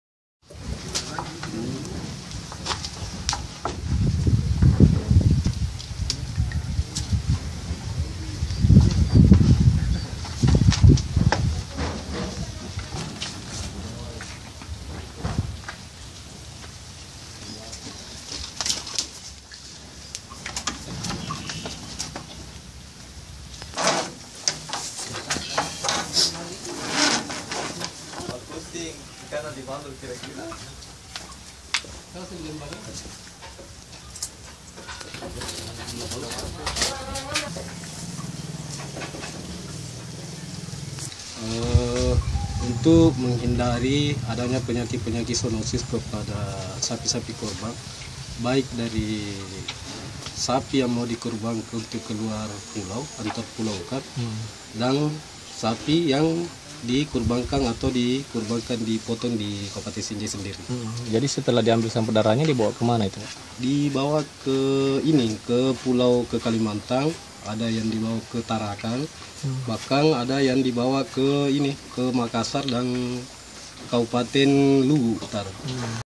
Jelang Idul Adha Sapi Qurban Divaksin di Sinjai
Menjelang Idul Adha di Sinjai Dinas Peternakan di Sinjai aktif vaksin sapi warga, Kamis (24/8/2017).